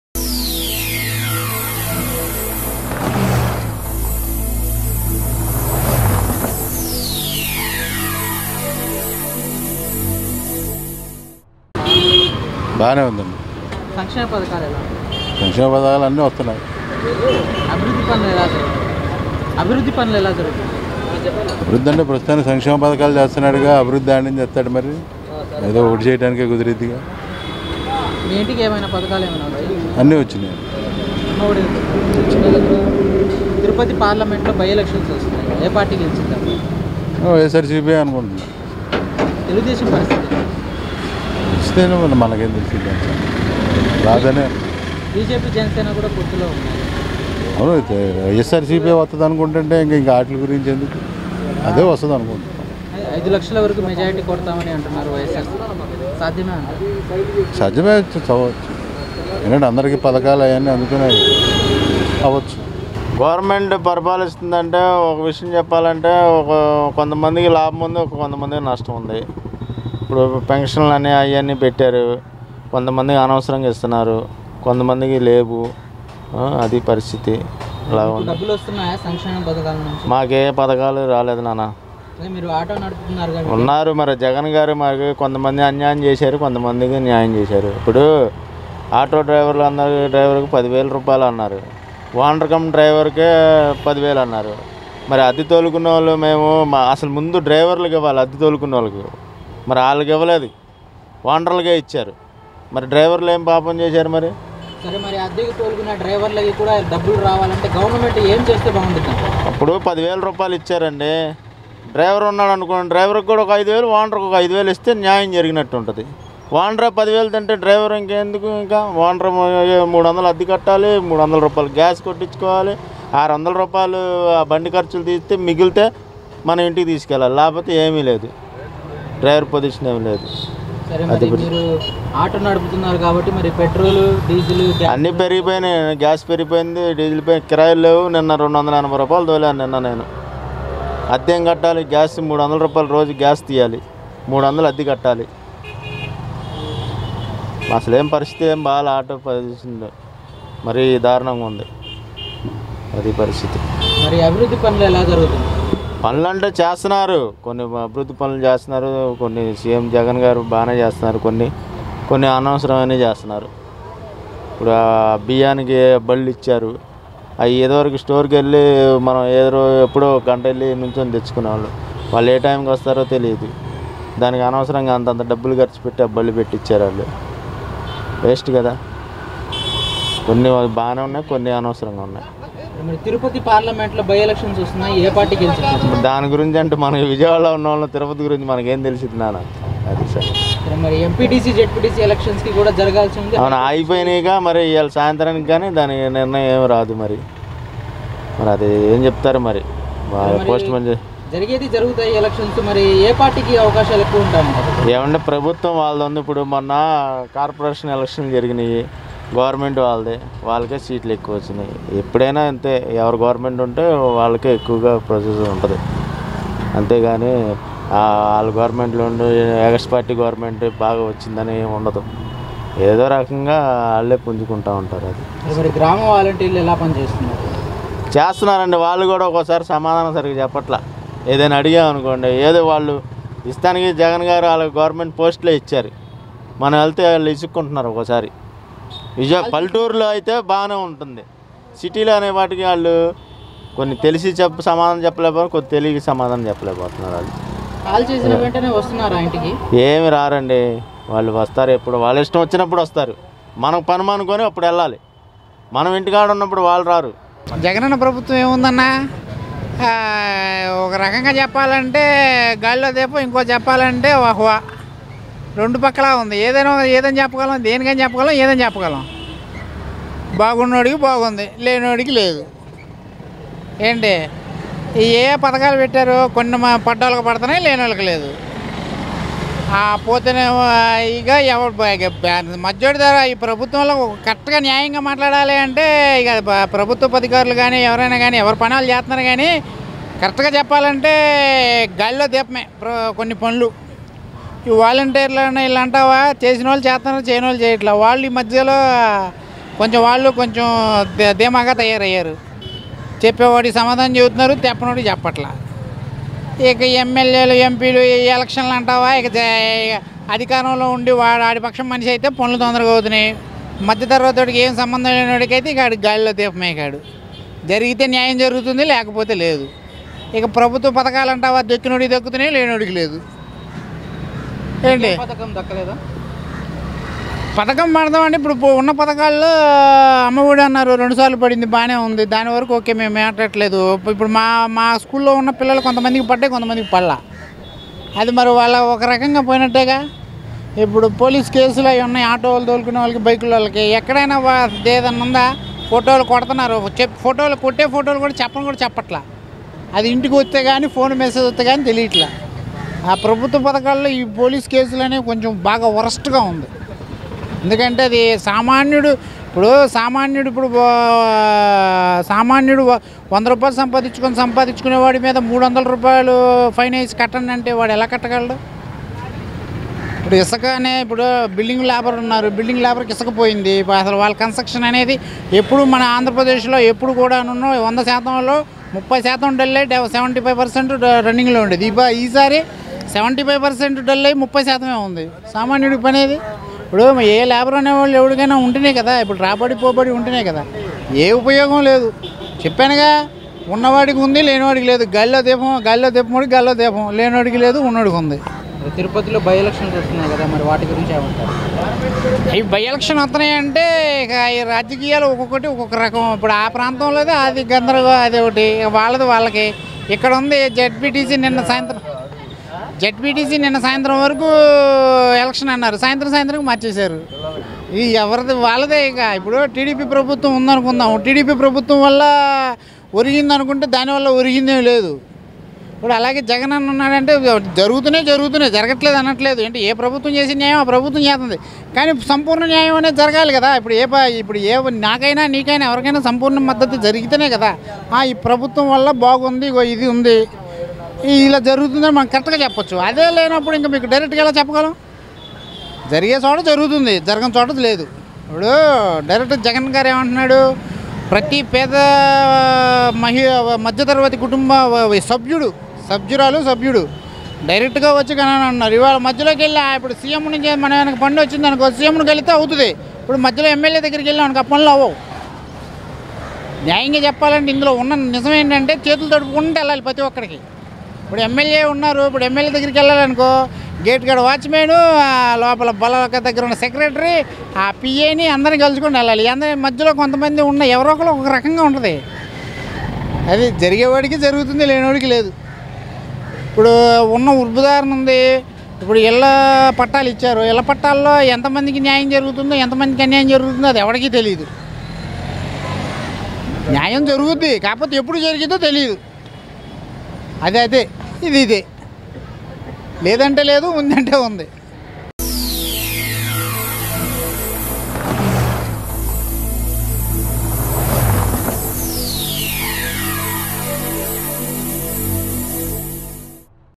Public Talk | YS Jagan | Chadrababu | ఈ ఆటో డ్రైవర్ల బాధలు వింటే జగన్ చలించిపోతాడు
ఈ ఆటో డ్రైవర్ల బాధలు వింటే జగన్ చలించిపోతాడు